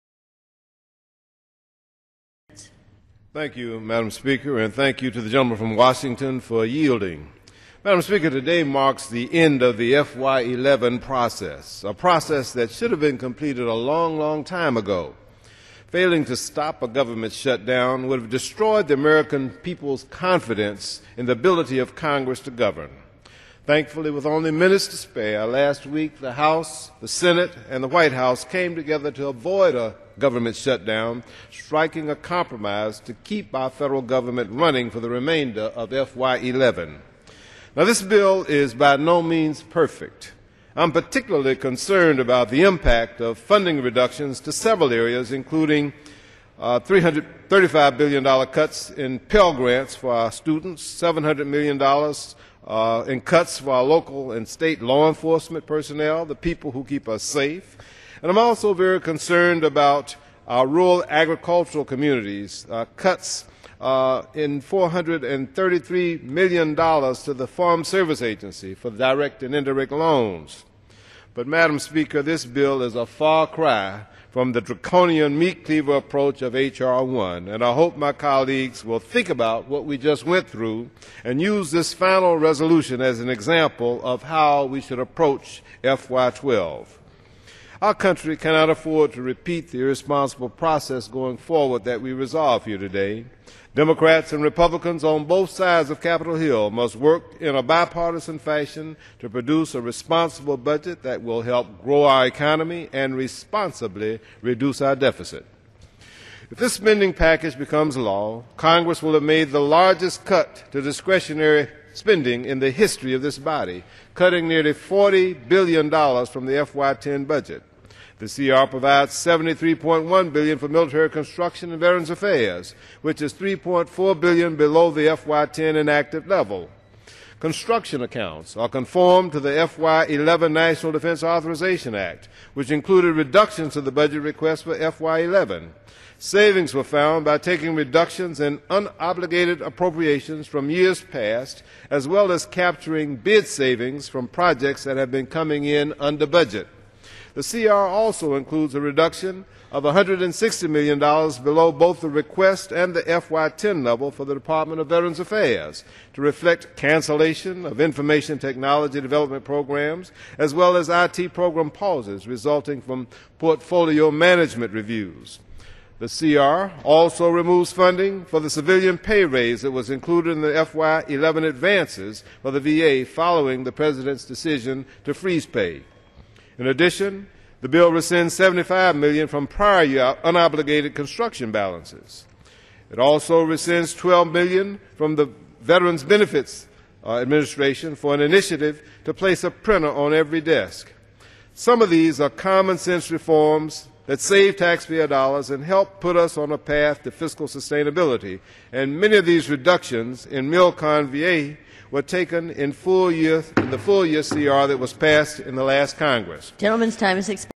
Rep. Sanford Bishop Floor Statement Regarding Responsible Budgeting
06.23.11

H.R.2219 DoD and Full-Year Continuing Appropriations Act of 2012